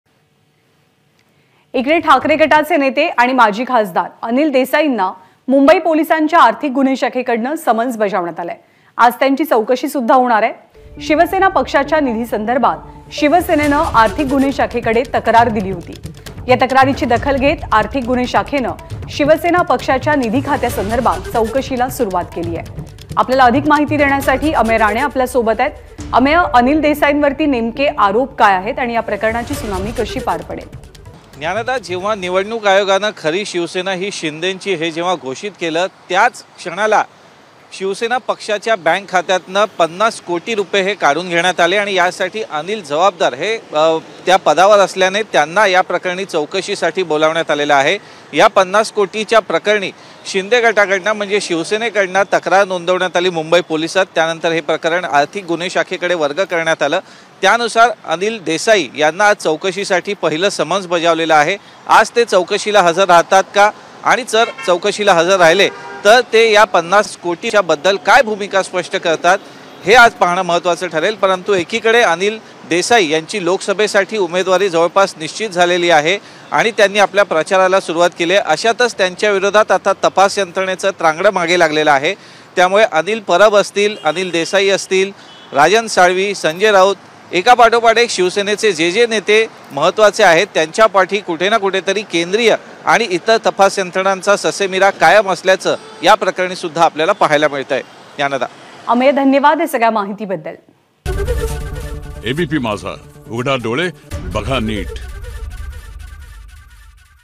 Anil Desai EOW Enquiry : अनिल देसाईंना आर्थिक गुन्हे शाखेकडून समन्स
आज त्यांची चौकशी केली जाणार आहे... शिवसेना पक्षाच्या निधीसंदर्भात शिवसेनेने आर्थिक गुन्हे शाखेकडे तक्रार दाखल केली होती या तक्रारीची दखल घेऊन आर्थिक गुन्हे शाखेने शिवसेना पक्षाच्या निधी खात्यासंदर्भातील चौकशी सुरू केलीये.. निवडणूक आयोगाने एकनाथ शिंदे यांची शिवसेना ही खरी शिवसेना असल्याचं जाहीर केल्यानंतर पक्षनिधी खात्यातील पन्नास कोटी रुपये काढल्याचा आरोप शिवसेना शिंदे गटाने करत तक्रार दाखल केली होती